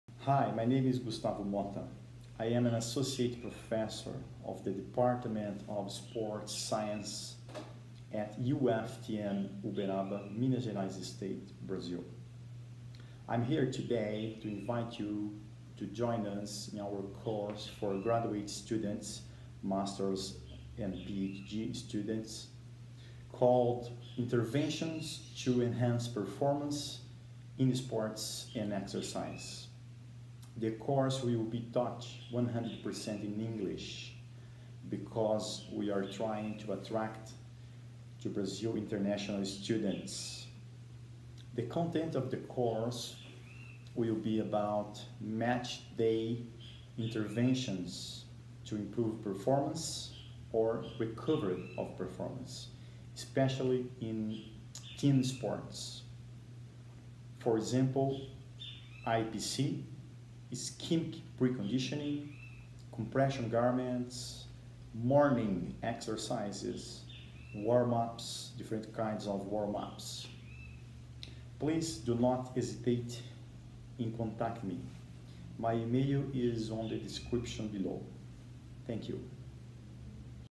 Course “Interventions to enhance performance in sports and exercise” at UFTM UBERABA Brazil
Invitation to graduate students to join us in our Course “Interventions to enhance performance in Sports and Exercise “ at UFTM Uberaba MG BRAZIL. UBERABA city is close to the border between Minas Gerais and the São Paulo States. It’s about 1-h flight from and to São Paulo, Campinas, Rio de Janeiro, Belo Horizonte and Brasília.

30 hours course; free of charges; just one week; 9 am to 11:30 - 1:30 - 4 pm (December 03rd to 07th, 2018).

Discipline content:
Match-day interventions to enhance the performance/recovery of team sports;  Research Design Concerns; Strategies such as: Ischemic preconditioning; Compression garments; Phototherapy; Warm-up; Morning exercise;
Post-activation potentiation;  Cold water immersion;